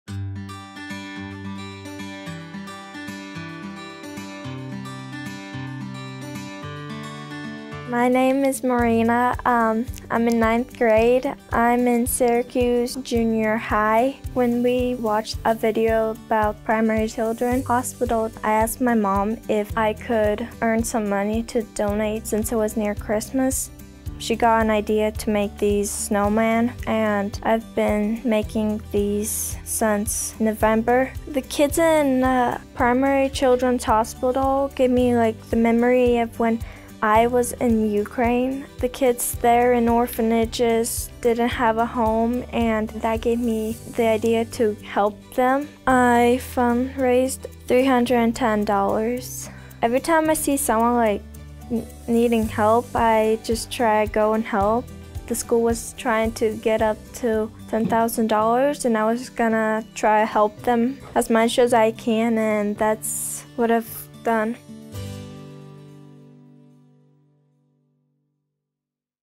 Syracuse Junior student raises money for Primary Children's Hospital
Syracuse Junior High's holiday fundraiser for Primary Children's Hospital sparked a personal project for one ninth grade student. After the school assembly where students saw a video about children at Primary Children's Hospital, Maryna went back to and encouraged everyone to donate.

"She told me that she was going to raise as much money as she could to donate to the fundraiser," said her teacher Melissa Heiner. "I told her that was a great idea, but I had no idea how much she would make."

Maryna went home and talked with her mom about the fundraiser and the impact the video had on her. Maryna was adopted at the age of 10 from Ukraine, and the video reminded her of the kids in the orphanage where she lived who needed help.

Maryna's mom suggested that she make something to sell, and that idea became a project of making little snowmen ornaments. Maryna personally made each ornament and sold them all at $2 a piece. With the money she collected from selling the ornaments and the other donations she collected, she was able to raise $310 by herself to donate to the school's fundraiser.

"Maryna is one of the kindest, caring people I have ever met," said Heiner. "Every day she goes out of her way to make others smile and to help them any way that she can. This world is a better place because Maryna is in it."